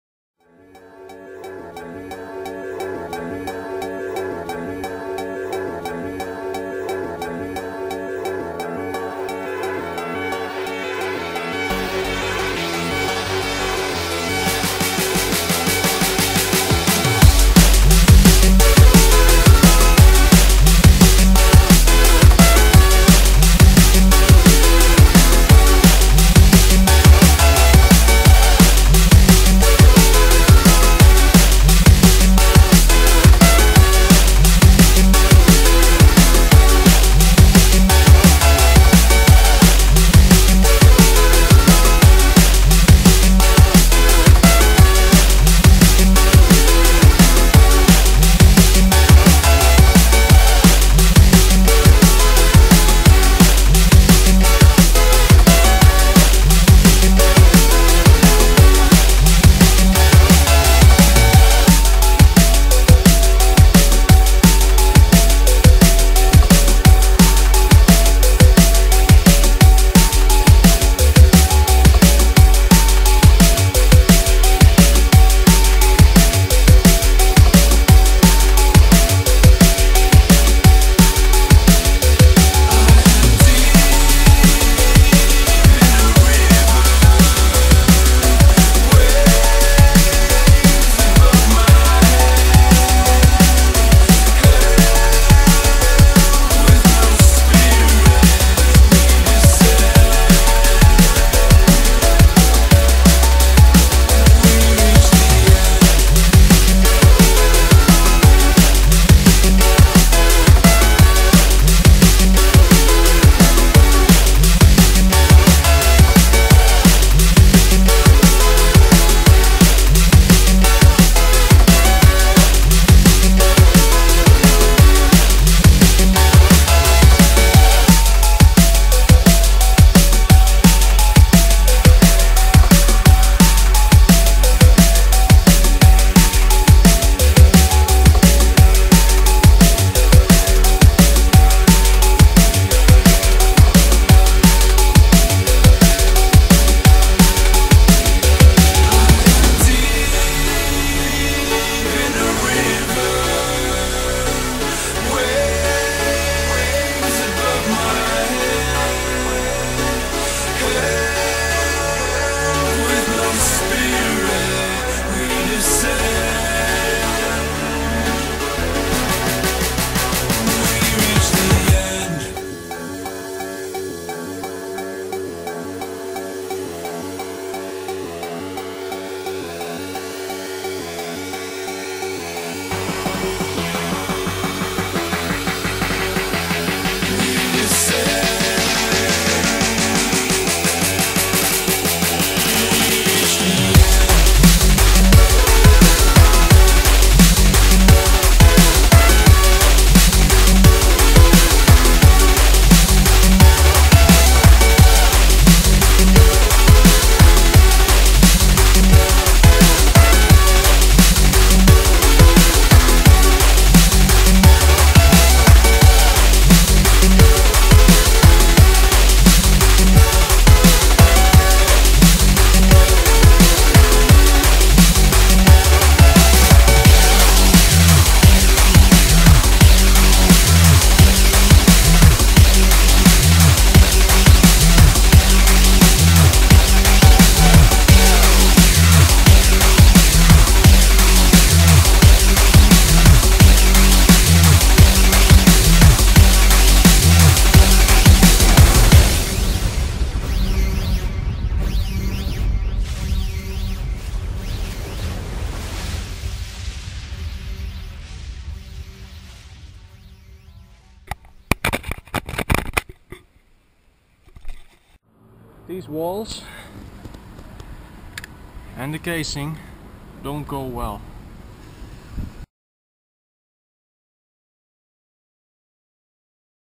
Together! - BB's Cam Test
So I finally got all the stuff: Camera and the chest-mount.
Started out to be a rainy day but overall wasn't something to complain about.

At the end of the day I scratched my camera casing which I wasn't so happy about.

Due to circumstances I wasn't able to edit the video with good software hence the quality.

Hope you like the video and let me know what you think!